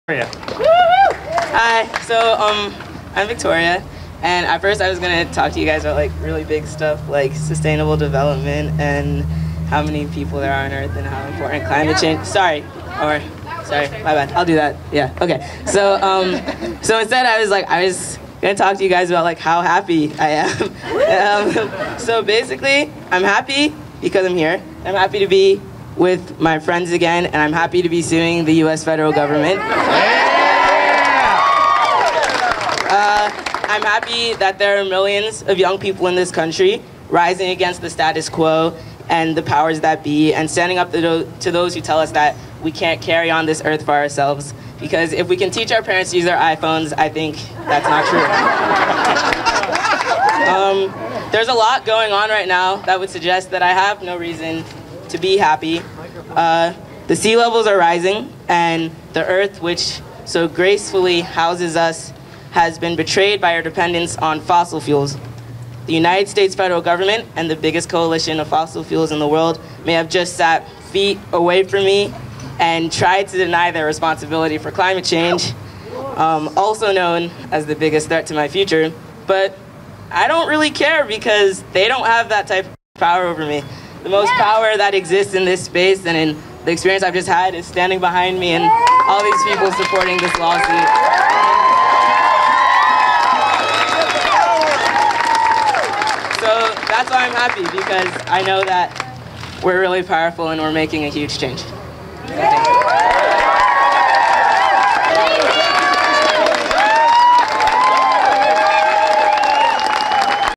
Victoria - Our Children’s Trust press conference following hearing before Judge Aiken
Twenty-one youth plaintiffs, and Dr. James Hansen, filed a lawsuit in 2015 against the United States, seeking to secure their constitutional and public trust rights to a stable climate. Three trade associations, representing the fossil fuel industry, intervened to protect their right to continue to pollute. Both the government and the trade associations filed motions to dismiss to throw the youth’s case out of
court. They lost. In April, U.S. Magistrate Judge Thomas Coffin recommended that all the motions to dismiss should be denied, paving the way for the case to move forward. However, both the government and fossil fuel industry objected to Judge Coffin’s findings and recommendations.

On Tuesday, September 13, 2016, U.S. District Court Judge Ann Aiken heard oral arguments on those objections from lawyers representing the parties.

This video is from a press conference on the courthouse steps following the hearing. Speakers at the press conference included:

Philip L. Gregory of Cotchett, Pitre & McCarthy, LLP.; Daniel M. Galpern, Esq.; Kelsey, Jacob, Jayden, Avery, Victoria, Alex, Journey, Xiuhtezcatl - youth plaintiffs in the case; and Julia A. Olson, Wild Earth Advocates, and Exec. Dir. & Chief Legal Counsel, Our Children’s Trust.

Video compliments of: TheClimateMobilization.org.

Find out more about Our Children's Trust at ourchildrenstrust.org